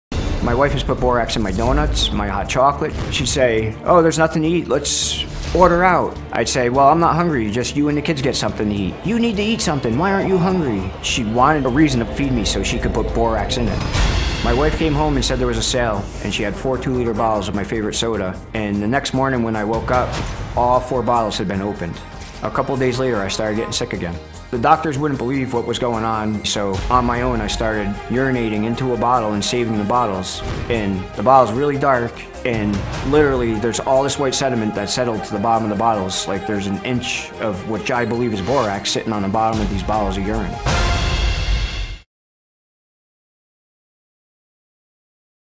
‘The Doctors Wouldn’t Believe What Was Going On’ Says Man Who Set Out To Prove His Wife Poisoned …
Jeremy says his doctors wouldn’t believe him when he accused his wife, Kelly, of poisoning him with borax, so he took measures to try to prove that’s what was going on.  Kelly adamantly denies poisoning Jeremy, and says his medical tests prove it.